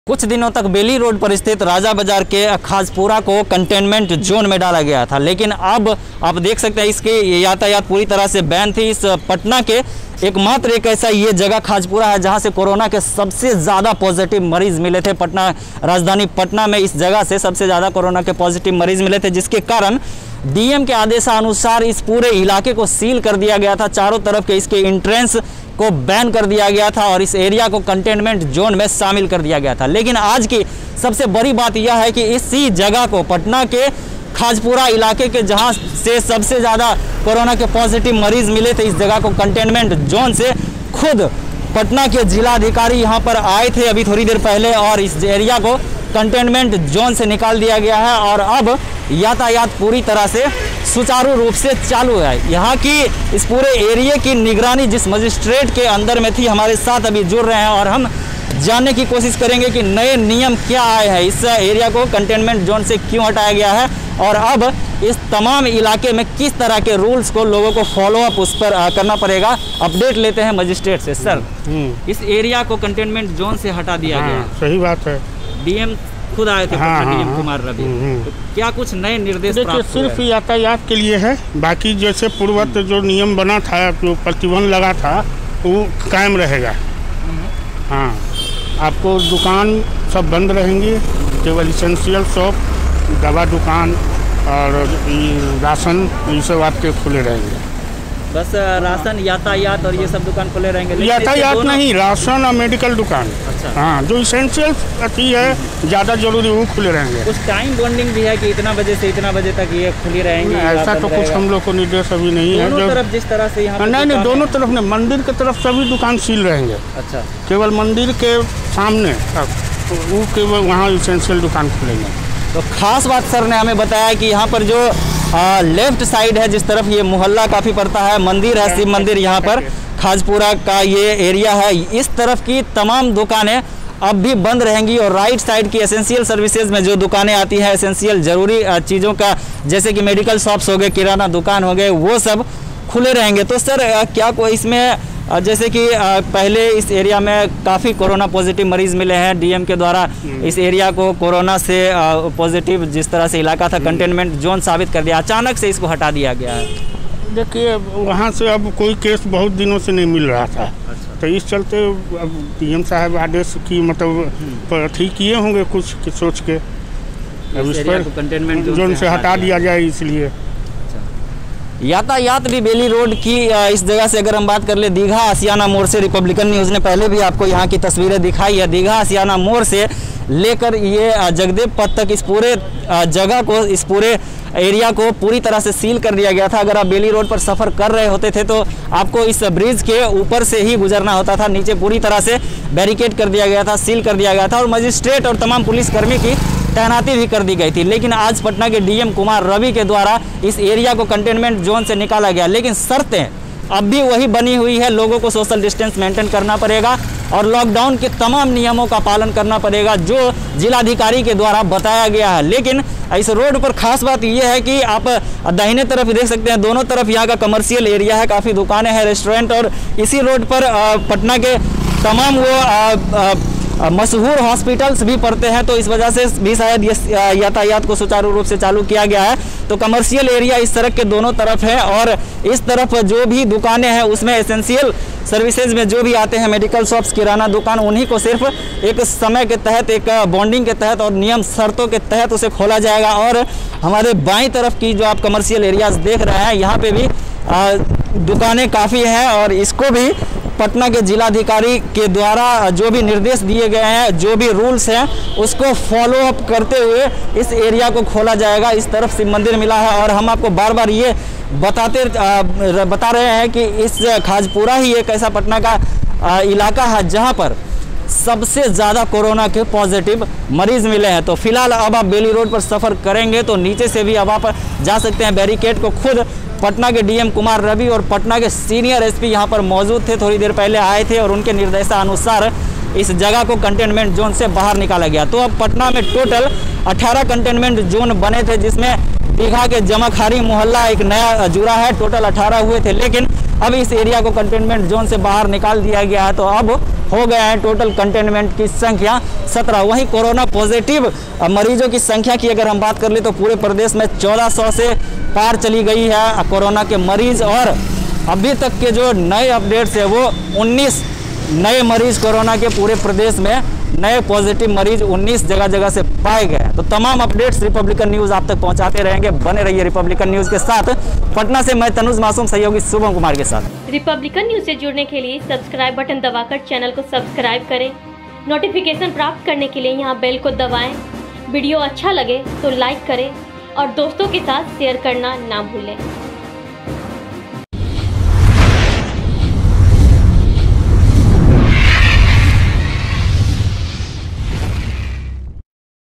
बड़ी खबर : Patna का ये इलाका Containment Zone से हुआ बाहर, DM का फैसला, मिले थे सबसे ज्यादा Positive
पटना का ये पॉश इलाका अब कन्टेनमेंट ज़ोन से हुआ बाहर। डीएम कुमार रवि का फैसला। यहां मिले से सबसे ज्यादा मरीज। देखिए बड़ी राहत के बाद का नजारा और क्या बोले मजिस्ट्रेट।